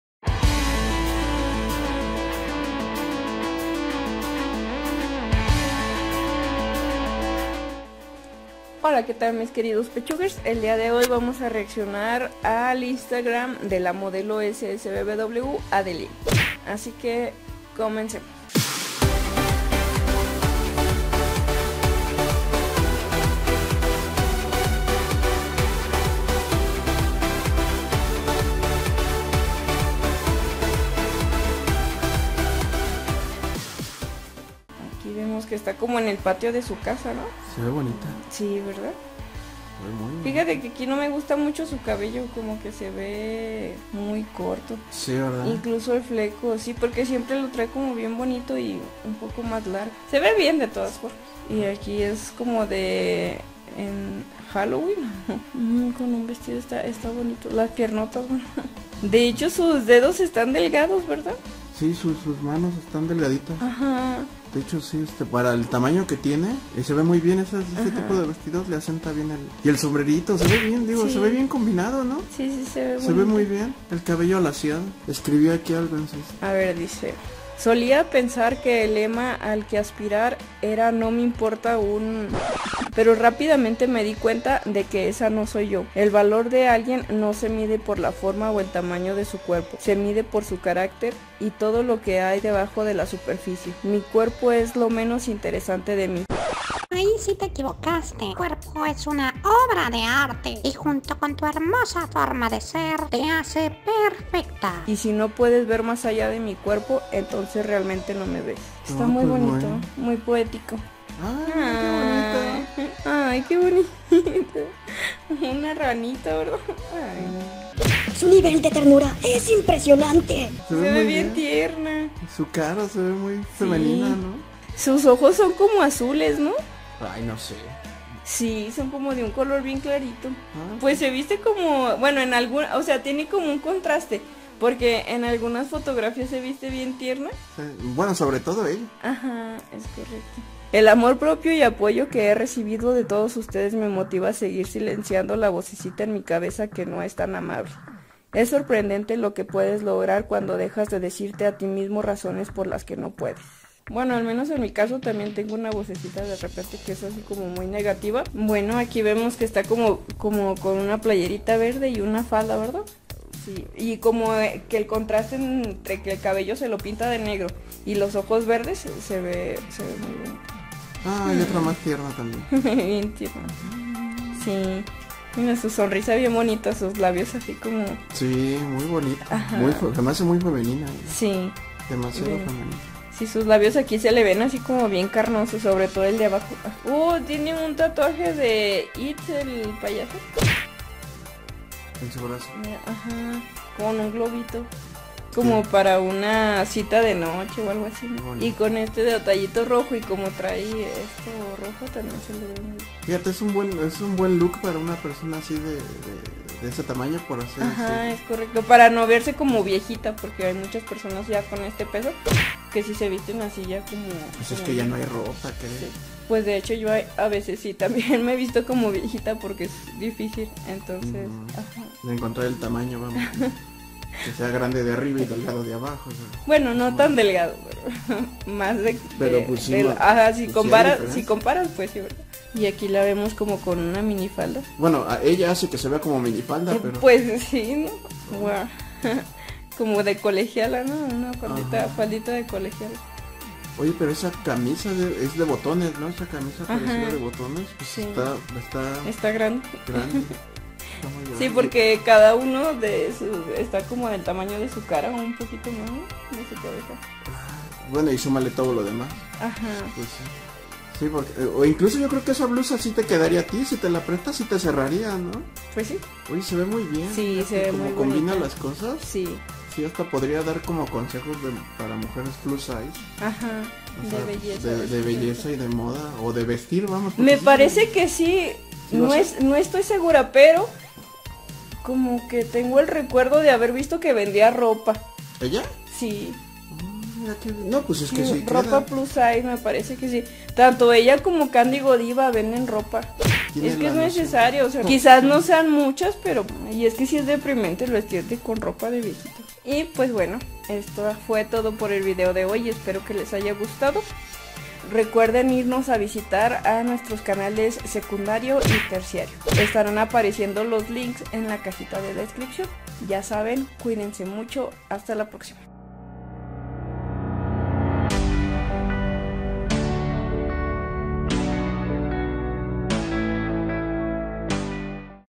Reaccionando al Instagram de Adeline SSBBW.
El día de hoy les traigo un video una modelo USSBBW, ella es Adeline. Nos metimos a su Instagram y reaccionamos a algunas cosas que ella ha publicado.

Con este vídeo no pretendo ofender a nadie, ni cambiar la forma de pensar de los demás, mi único y seguro fin de hacer esto es: entretener a la audiencia.